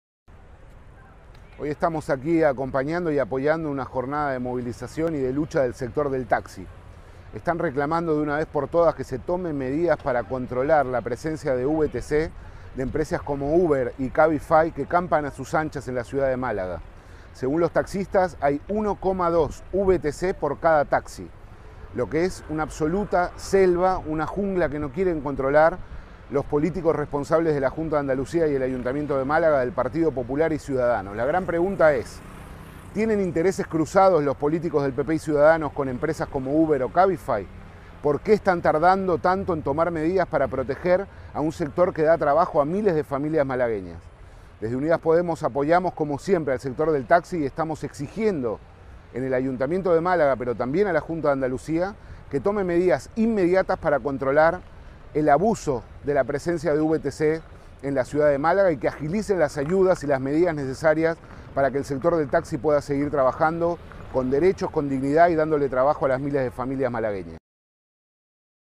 Miles de taxistas se manfiestan en Málaga contra la intrusión de los VTC
Nico Sguiglia, concejal de Podemos en el Ayuntamiento de Málaga

Los taxistas exigen al Ayuntamiento de Málaga y a la Junta de Andalucía que acabe con la competencia desleal y el intrusismo profesional en los que incurren las empresas de Vehículos de Transporte con Conductor (VTC), como son Uber y Cabify.

Los representantes del sector han ofrecido una rueda de prensa a las puertas de María Zambrano, junto al portavoz del PSOE, Daniel Pérez, y a su homóloga en Unidas Podemos, Paqui Macías. Desde la estación y hasta el Ayuntamiento ha partido una enorme caravana de taxis que ha rodeado el consistorio, además de ocupar parte de los carriles del Paseo del Parque.